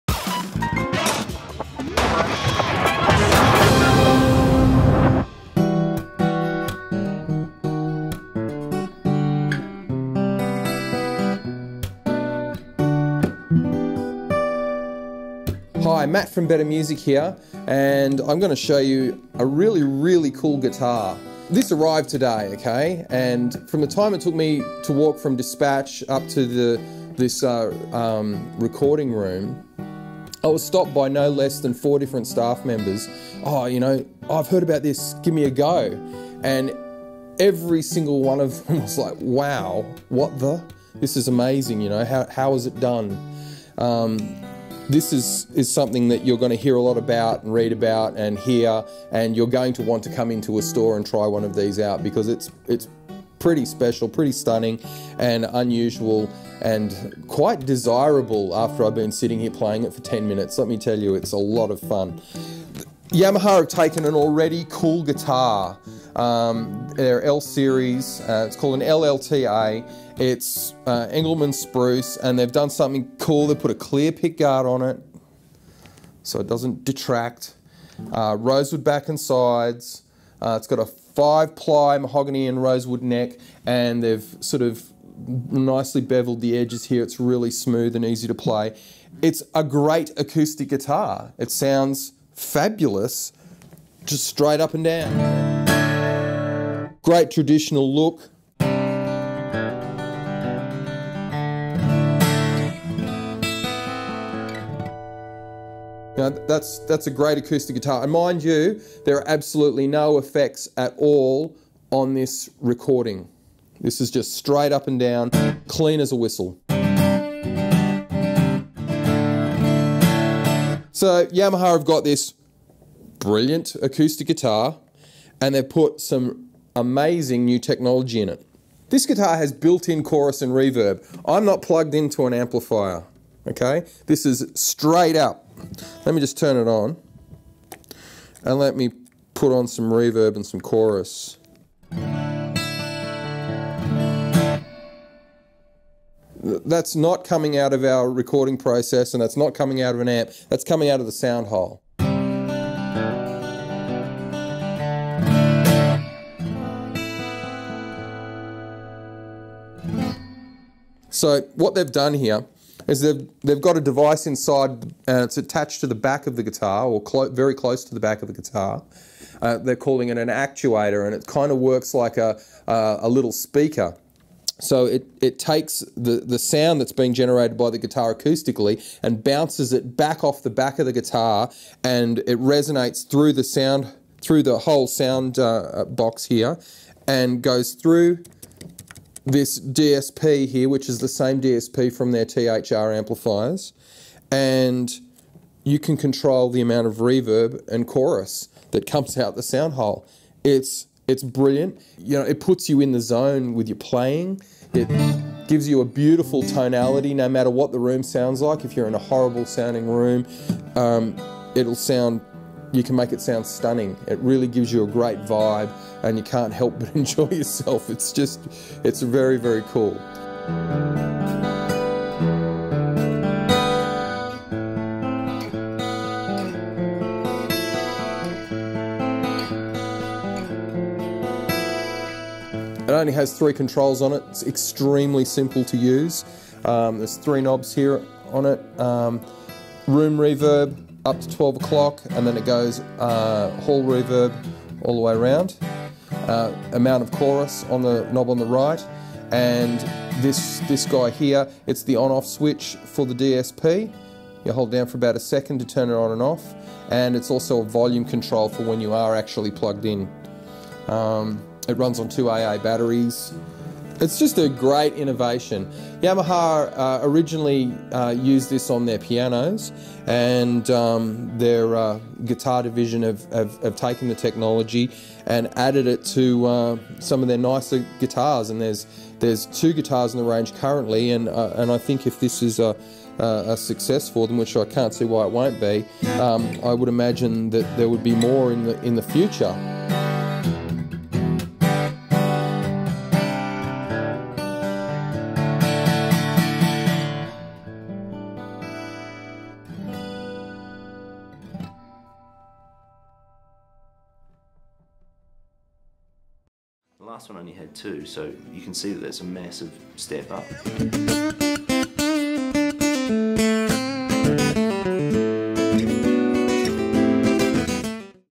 Yamaha LL-TA TransAcoustic Guitar | Better Music
Matt found a guitar that makes him feel like a little kid again... The brand new TransAcoustic from Yamaha.